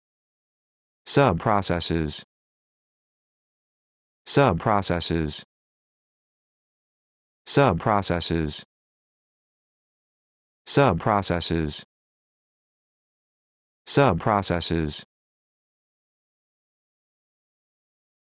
How to Pronounce Subprocesses
This video shows you how to pronounce Subprocesses